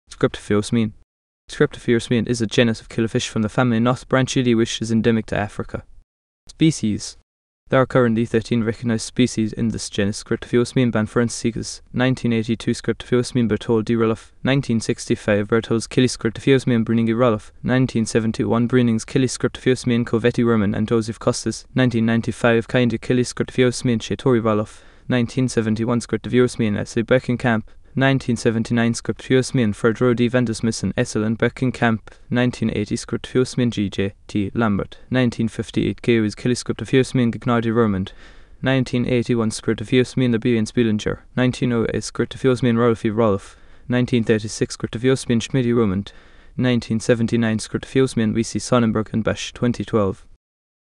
Scriptaphyosemion
Scriptaphyosemion is a genus of killifish from the family Nothobranchiidae which is endemic to Africa.